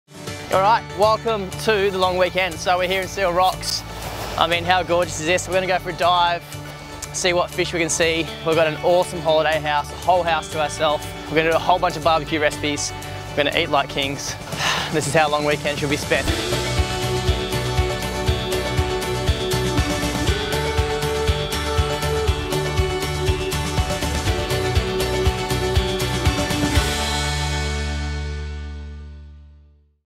The Long Weekend - Mojo Pork Recipe Teaser
Some food inspiration for your next weekend away - mojo marinated pork chops with avocado and pineapple salsa. Dive, swim and surf the day away then come home for a superb dinner with friends shared over your holiday house table.